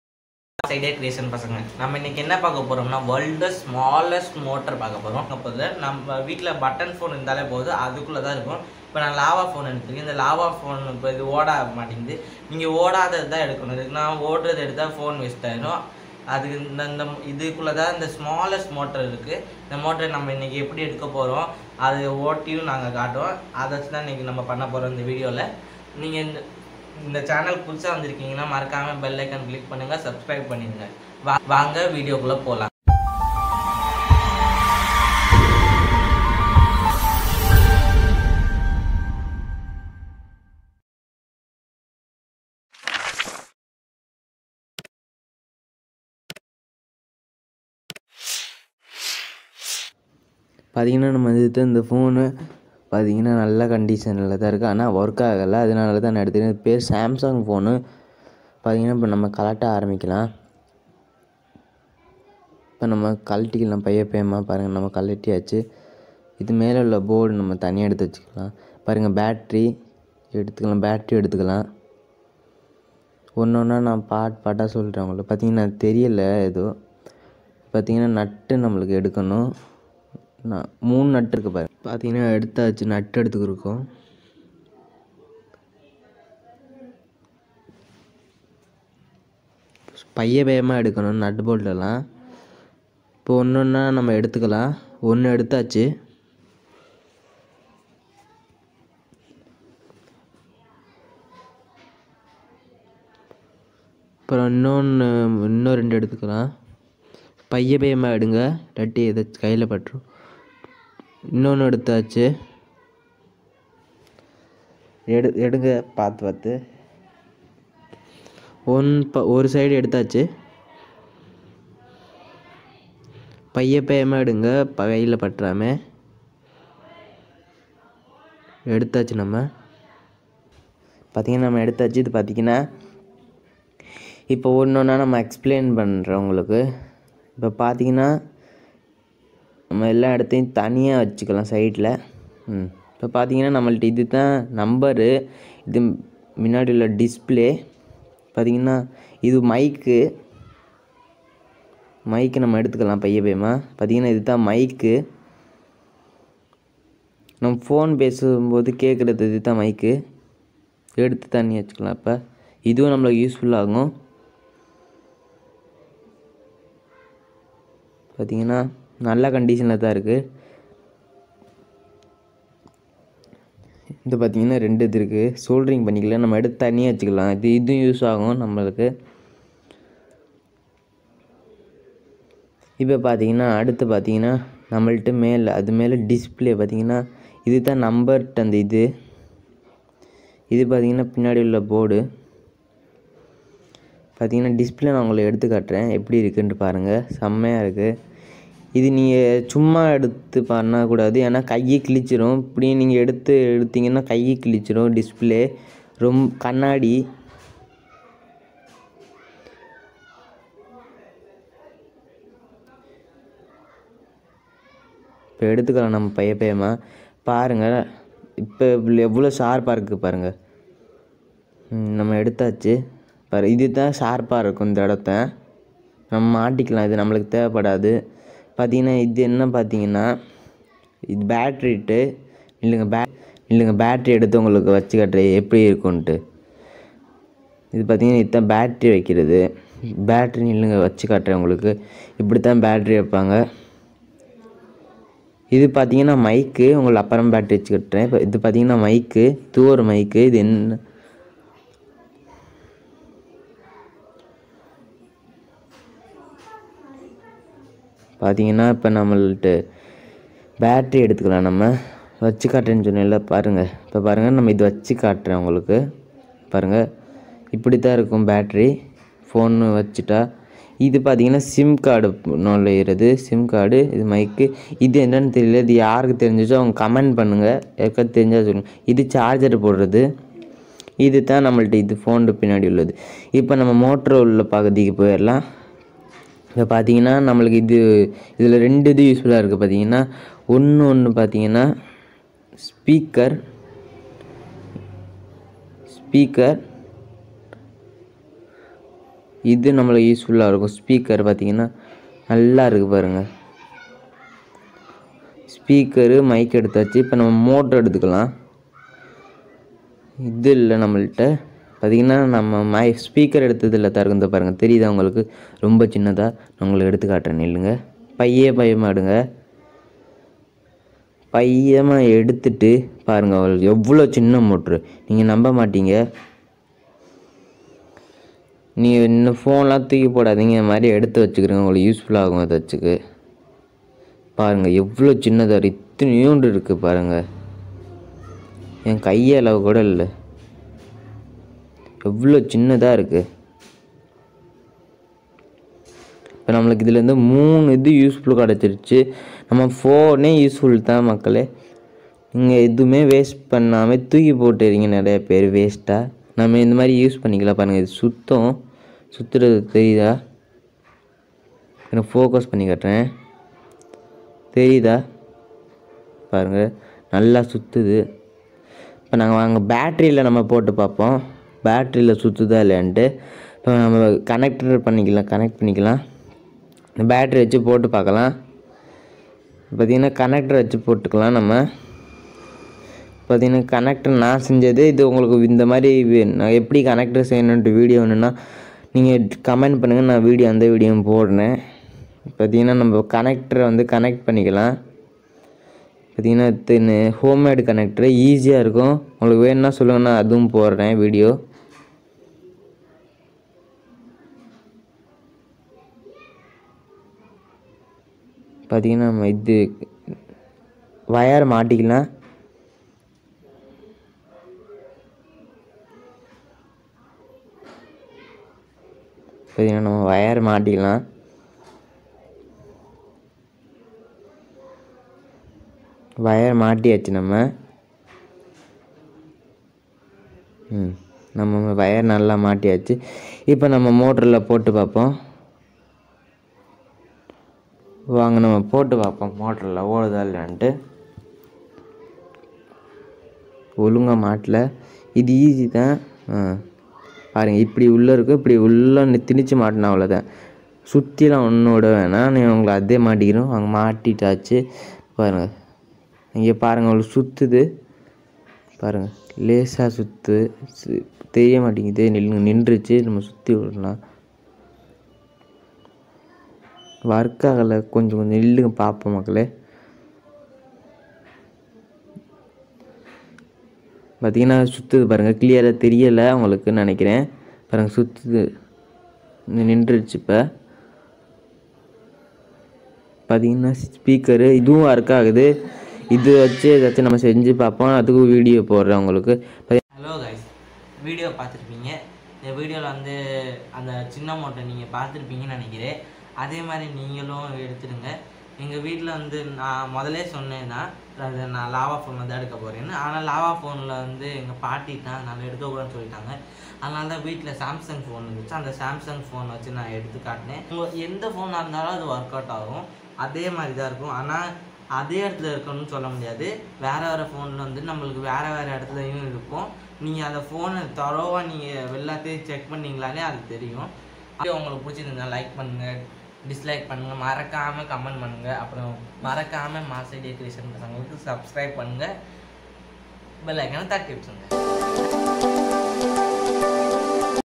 SMALLEST🤏MOTOR || INTO THE BUTTON PHONE😱 || TAMIL ||Vera level|| # TWIN FACTORY
HLLO FRIENDS

welcome to mass idea creation pasanga
today we are going to 👀see smallest motor
into normally button phone😱
 so much entertainment omg